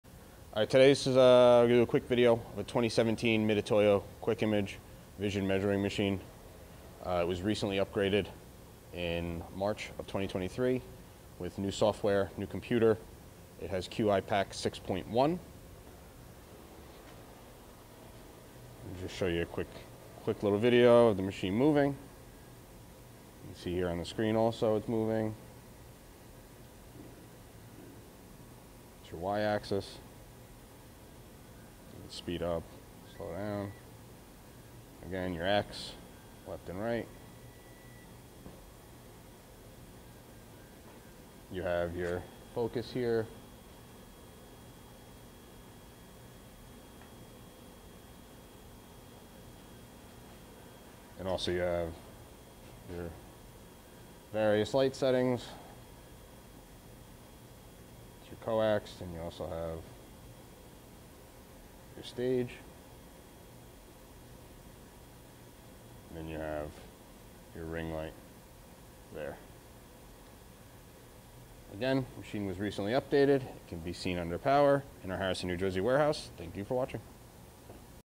Mitutoyo QL2010D Manual 2-D Vision Measuring Machine
Mitutoyo QL2010D Manual 2-D
 Vision Measuring Machine

S/N:0605004, New 2017

MEASURING RANGE (XxY):. 7.87" x 3.94"(200 x 100 mm)
EFFECTIVE STAGE GLASS SIZE:. 9.53" x 5.51" (242 x 140 mm)
MAXIMUM STAGE LOADING:. APPROX. 22 LBS(10 KG)
MAIN UNIT MASS:. APPROX. 158 LBS (72 KG)
VIEW FIELD:. ….. 1.26" x 0.94" (32 x 24 mm)
MEASUREMENT MODE:. HIGH RESOLUTION MODE/NORMAL MODE
TRAVEL RANGE (Z AXIS):. 3.94" (100 mm)

MEASUREMENT ACCURACY WITHIN THE SCREEN
HIGH RESOLUTION MODE:. +/- 2 um
NORMAL MODE:. +/- 4 um

REPEATABILITY WITHIN THE SCREEN
HIGH RESOLUTION MODE:. +/- 1 um
NORMAL MODE:. +/- 2 um
MEASUREMENT ACCURACY:. +/-(3.5+0.02L)um L:ARBITRARY MEASURING LENGTH (mm)
MONITOR MAGNIFICATION:. 7.6X
MAGNIFICATION:. 0.2X TELECENTRIC OPTICAL SYSTEM
DEPTH OF FOCUS HIGH RES.:. +/- 0.6 mm
DEPTH OF FOCUS NORMAL MODE:. +/- 11 mm
WORKING DISTANCE:. 3.54" (90 mm)
CAMERA:. 3 MILLION PIXELS, 1/2" FULL COLOR

ILLUMINATION:
  TRANSMITTED LIGHT" GREEN LED TELECENTRIC ILLUMINATION
  CO-AXIAL LIGHT: WHITE LED
  RING LIGHT: 4-QUADRANT WHITE LED

POWER SUPPLY 100-240 VAC 50/60 HZ
ACCURACY GUARANTEED TEMPERATURE RANGE:  19-21 DEGREES C

** "AS NEW" CONDITION & APPEARANCE **
** SHIPS FROM OUR HARRISON, NJ WAREHOUSE **